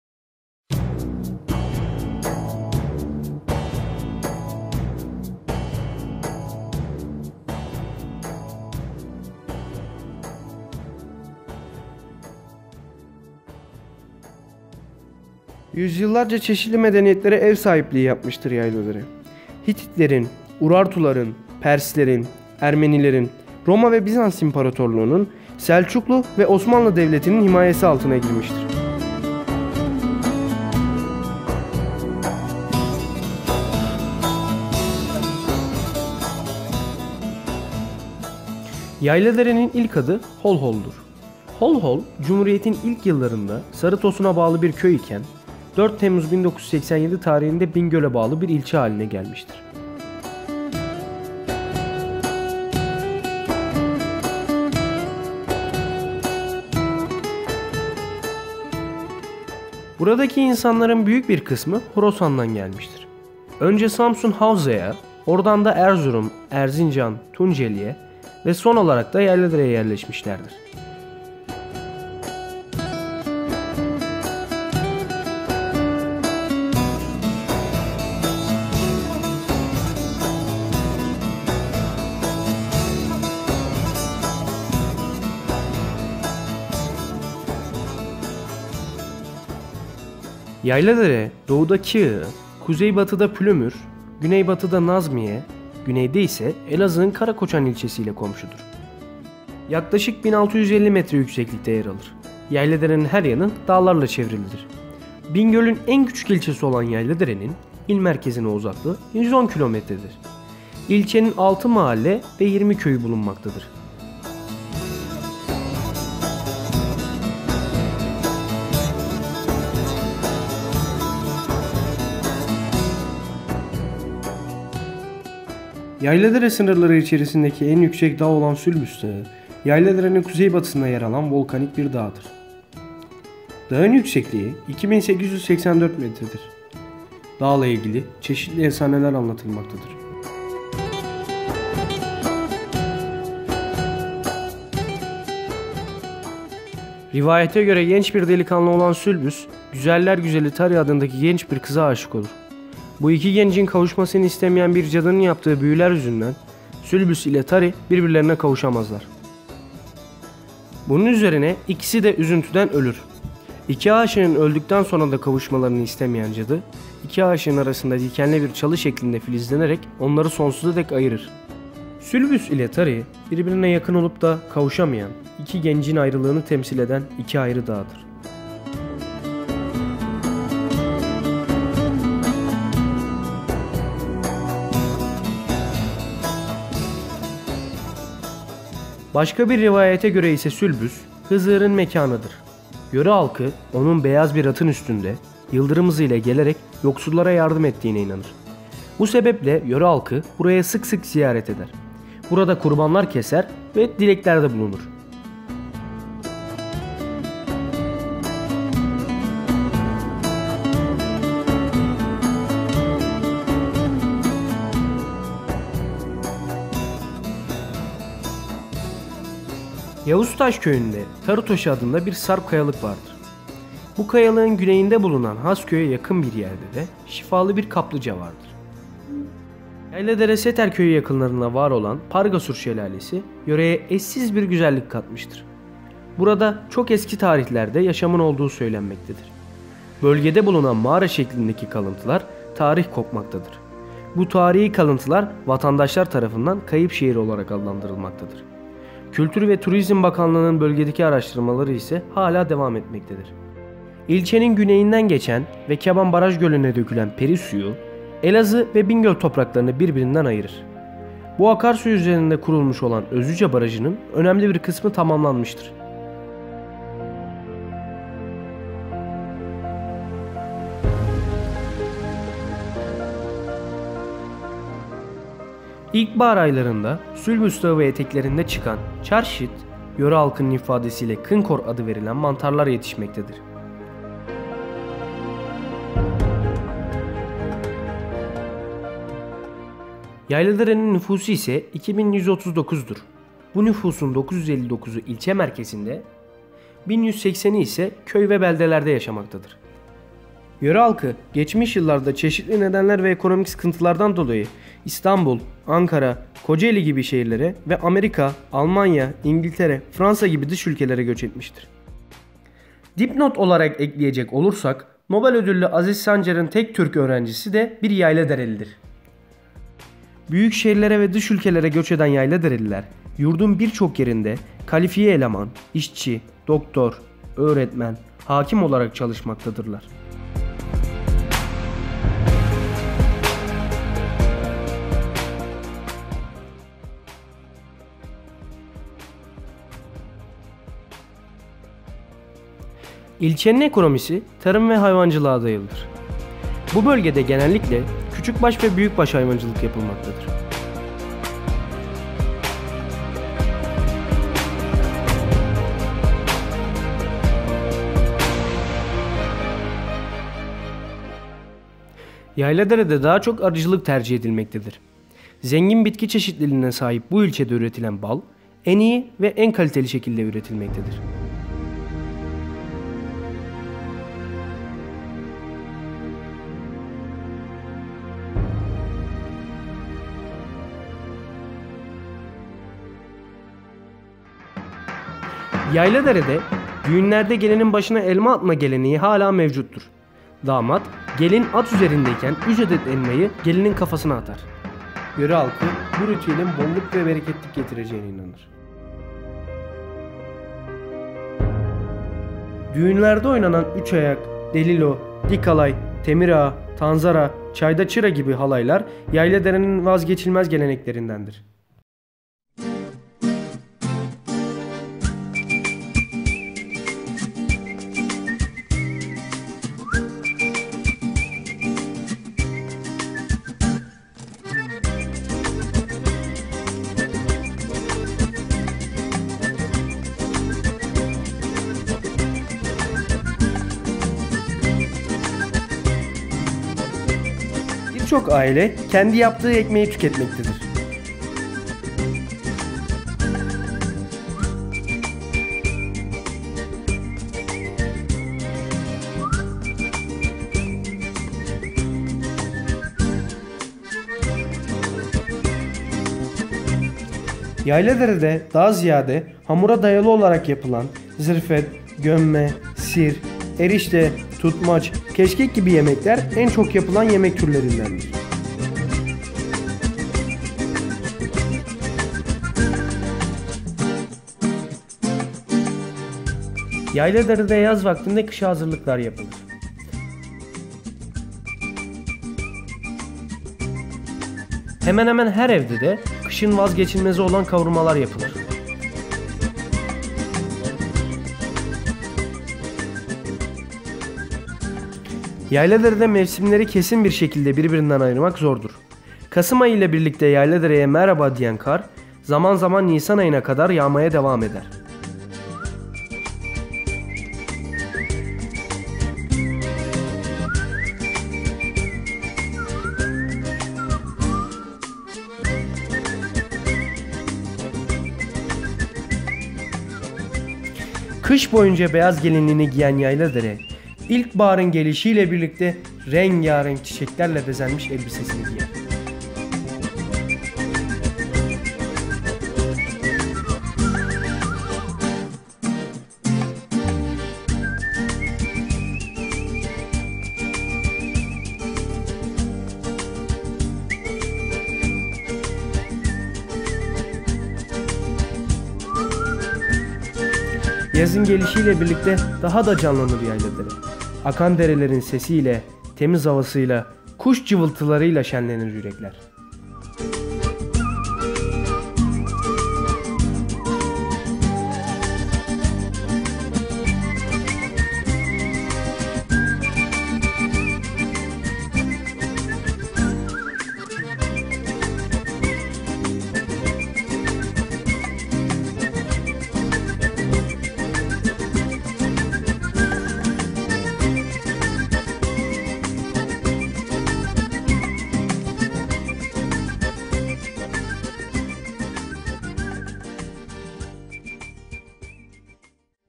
Yayladere Belgeseli- Yayladere Tanıtım Belgeseli(4006 Tübitak Bilim Fuari Kapsamında hazırlanmıştır)
Danışman Öğretmen
Meryem Bozaba
Görevli öğrenciler
Eray Yılmaz
Murat Kartal
4006 tübitak
Bingöl Yayladere